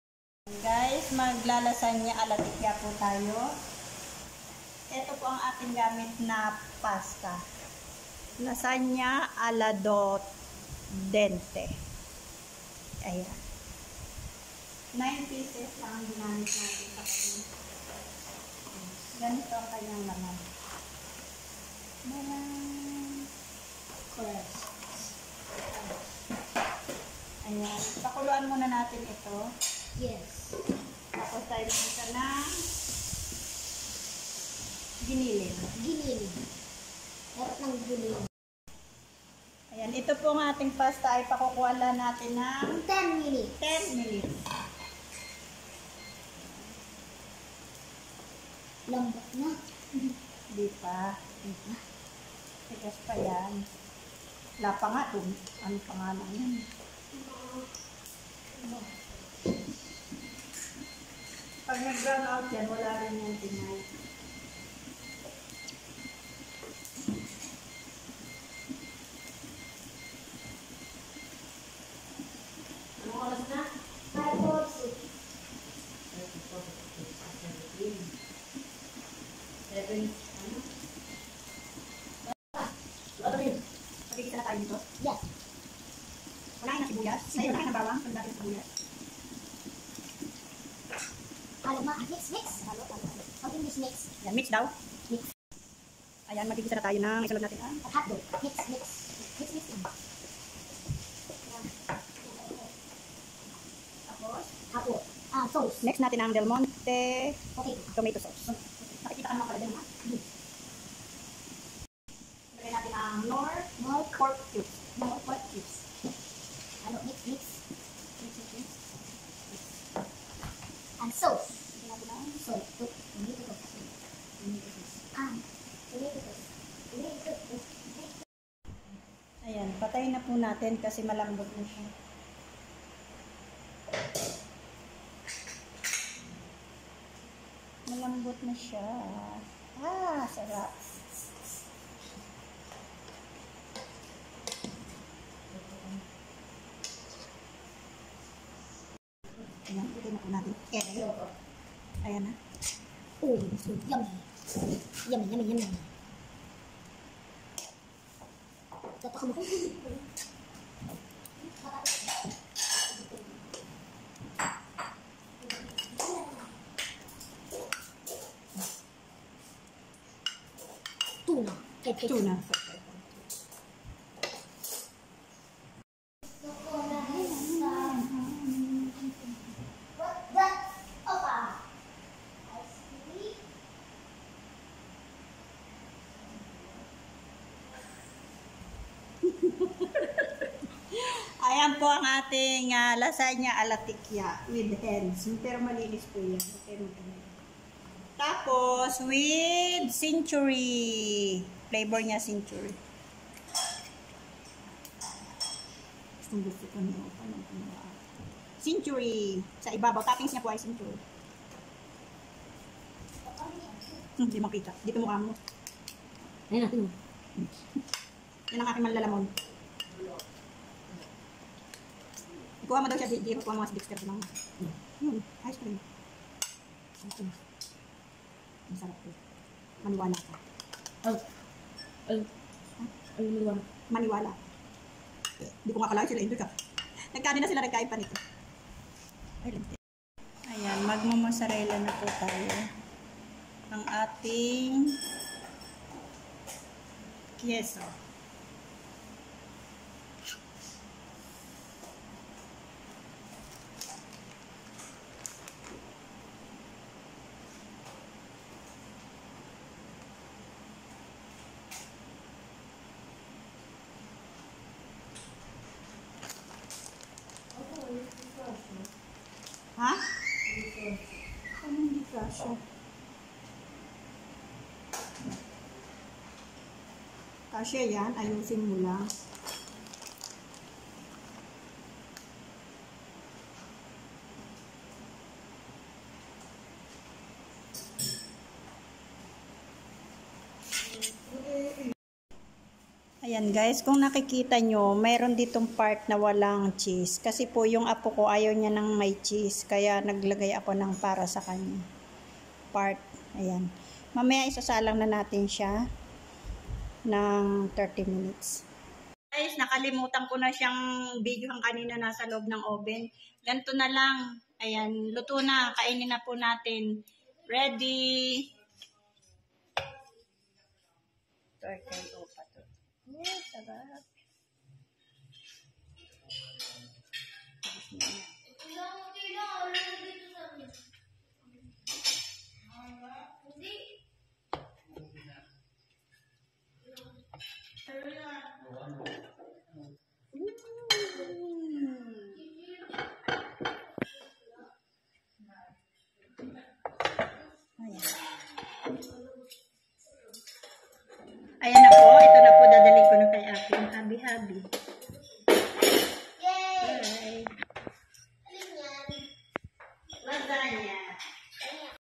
Simple Lasagna recipe | Madam Tikya Ang Biyahera
Lasagna recipe pabg masa.
 1/2 kl giniling baboy
6 pcs
2 can century tuna
2 butil ng bwang
1 pc sibuyas katamtaman ang laki
1 pc knor cube pork
1 table spoon oil